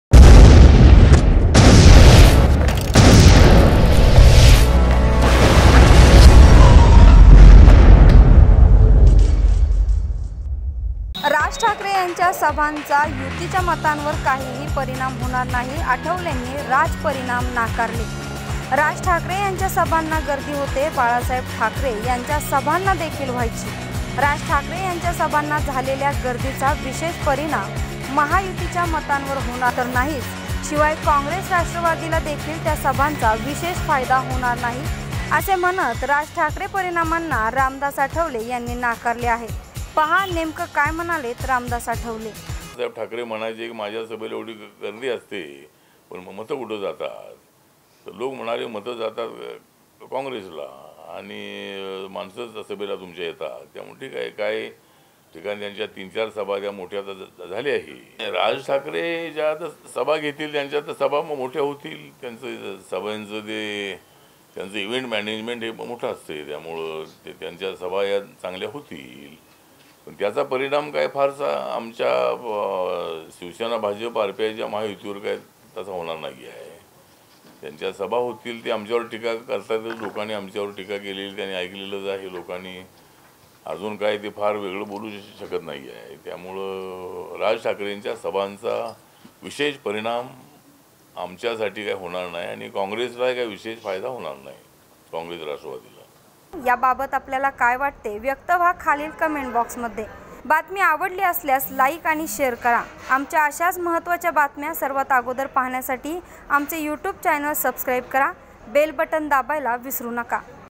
राज ठाकरे यांच्या सभांचा युतीच्या मतांवर काहीही परिणाम होणार नाही; आठवलेंनी राज परिणाम नाकारले
पुणे, दि.१० (चेकमेट टाईम्स): राज ठाकरे यांच्या सभांना गर्दी होते, बाळासाहेब ठाकरे यांच्या सभांना देखील व्हायची. राज ठाकरे यांच्या सभांना झालेल्या गर्दीचा विशेष परिणाम महायुतीच्या मतांवर होणार तर नाहीच, शिवाय कॉंग्रेस राष्ट्रवादीला देखील त्या सभांचा विशेष फायदा होणार नाही असे म्हणत राज ठाकरे परिणामांना रामदास आठवले यांनी नाकारले आहे. पहा नेमकं काय म्हणालेत रामदास आठवले...